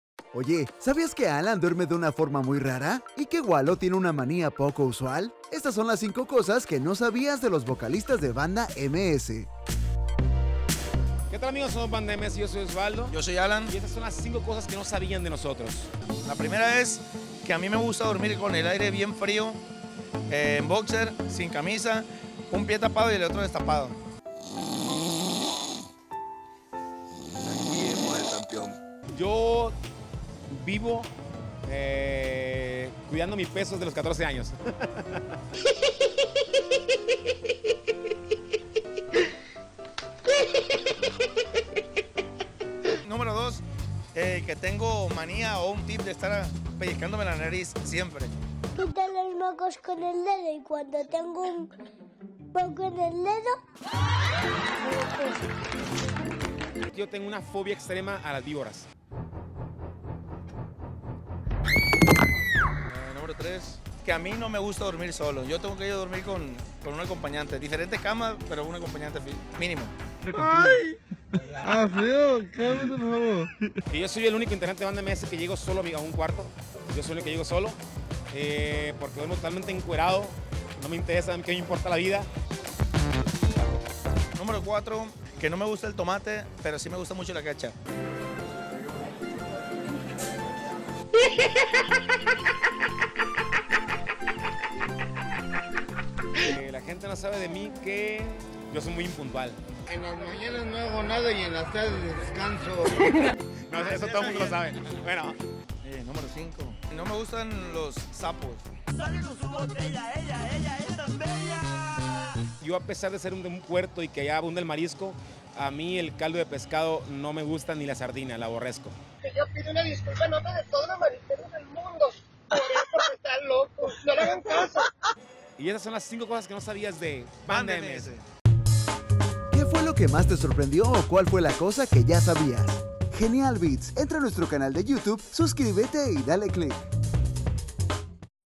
VIDEOS BANDA MS
En este video te mostraremos las 5 cosas que no sabias de los Alan Ramirez y Walo Silvas, los vocalistas de La Banda MS. De una forma muy divertida los cantantes, de la que es la banda del momento, nos abren su corazon y nos revelan las cosas que la gente desconoce de ellos. Desde su forma rara de dormir hasta sus fobias. Aqu iestan las 5 cosas que no sabias de Walo y Alan, vocalistas de la Banda MS.